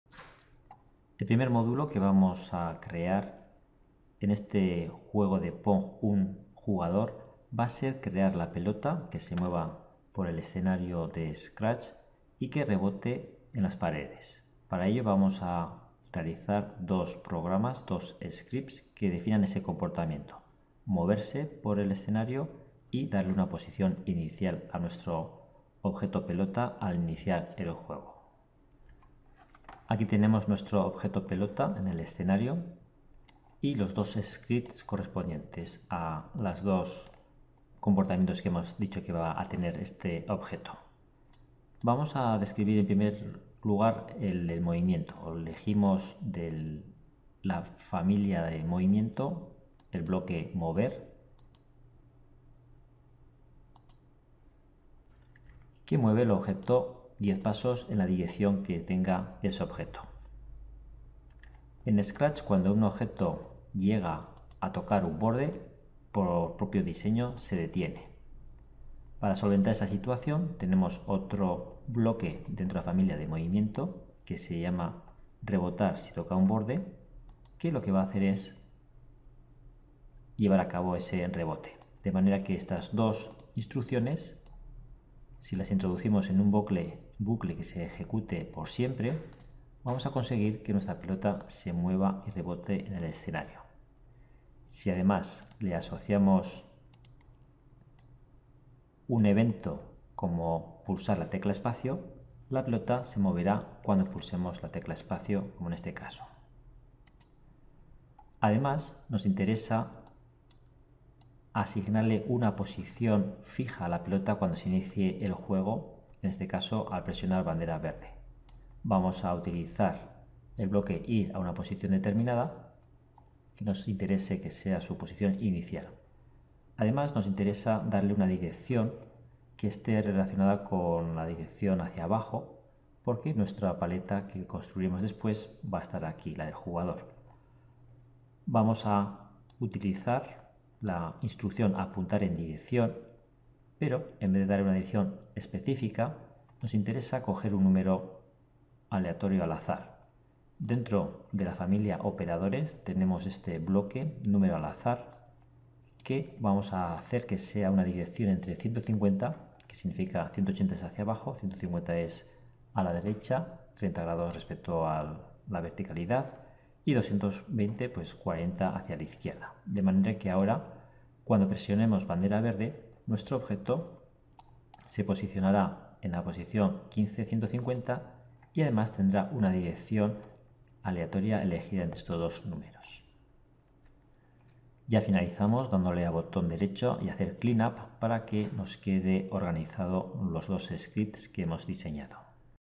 2. 4 - Crear Pelota
Vídeo incluido en el curso "Pensamiento Computacional en la Escuela" disponible en Miriada X

Objeto Pelota:

• movimiento por el escenario

• posición inicial